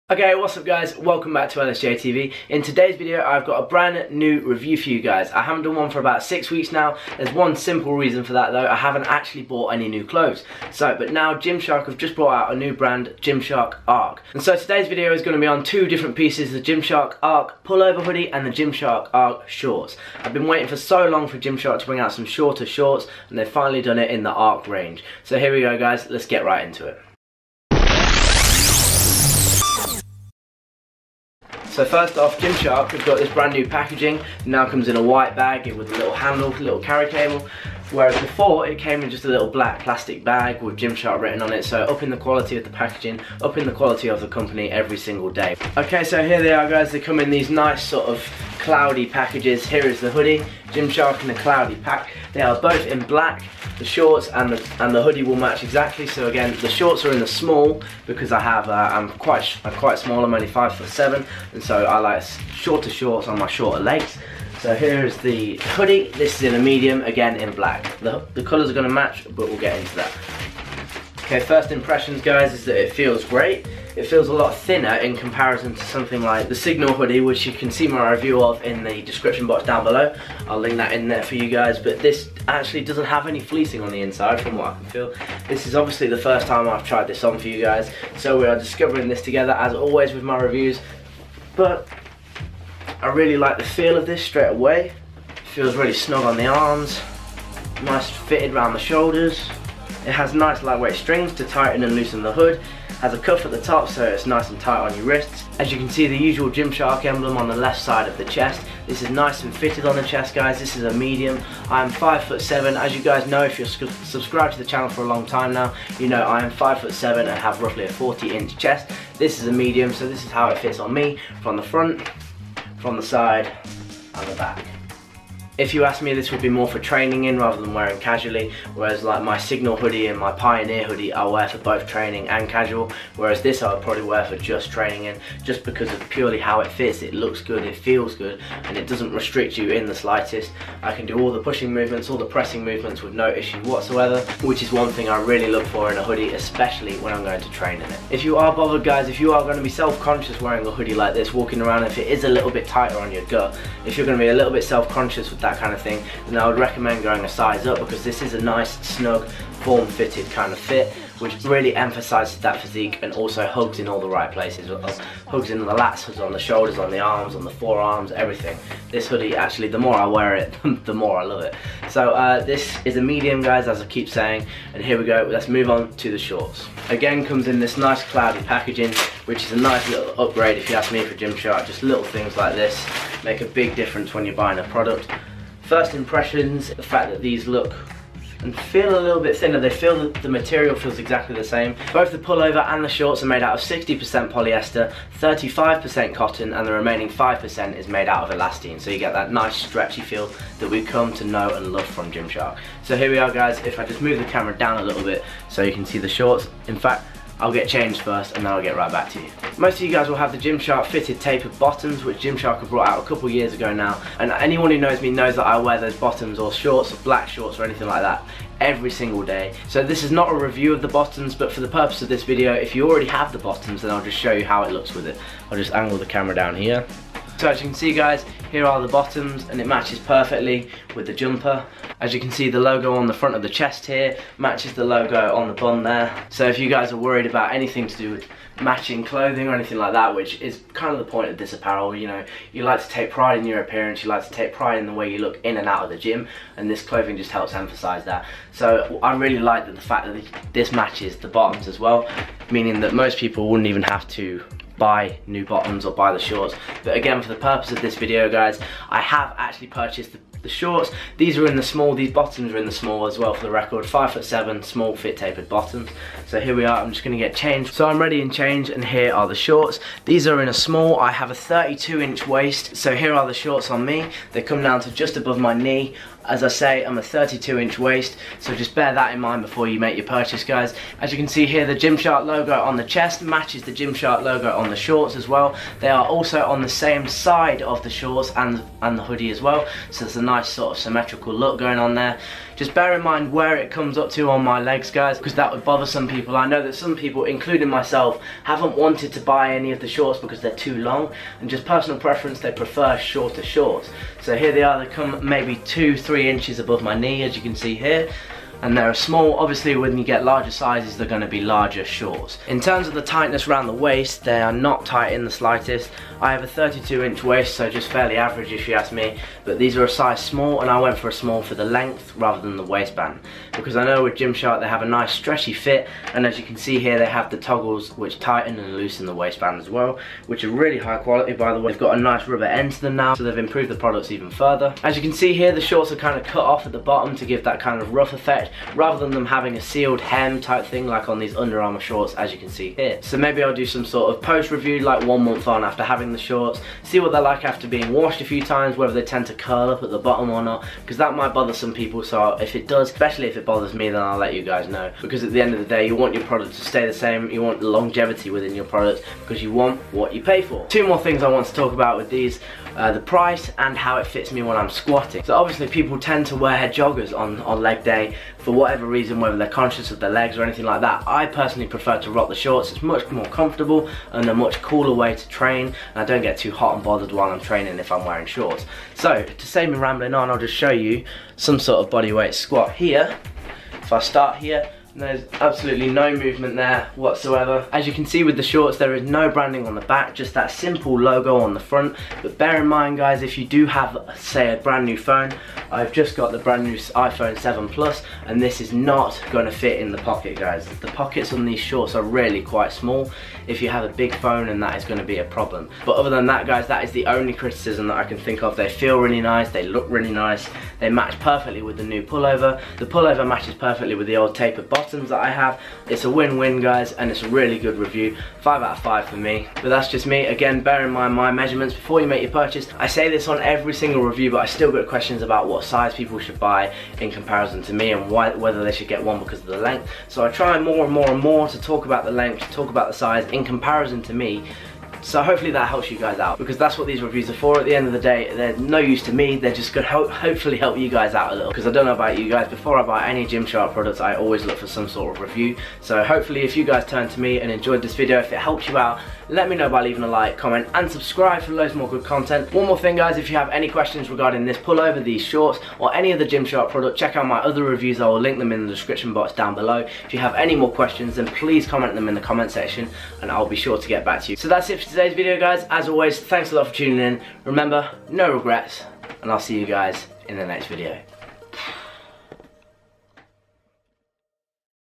Gymshark "Ark" Full Range Review
Enjoy the BRAND NEW Gymshark Ark Clothing Review! Any questions please put them in the comments section i'll be sure to get back to you!